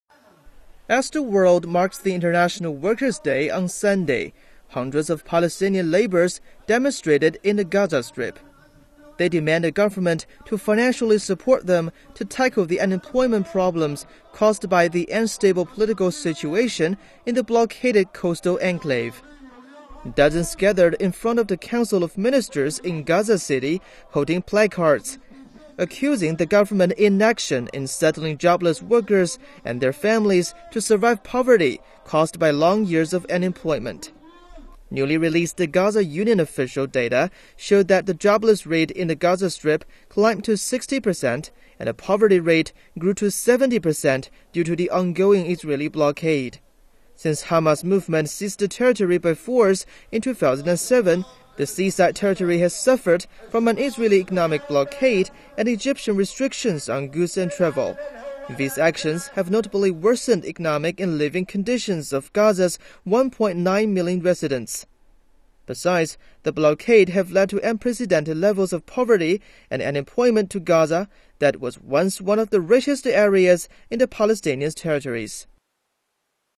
Palestinian workers protest on May Day
As the world marks the international Workers' Day on Sunday, hundreds of Palestinian labors demonstrated in the Gaza Strip.

They demand the government to financially support them to tackle the unemployment problems caused by the unstable political situation in the blockaded coastal enclave.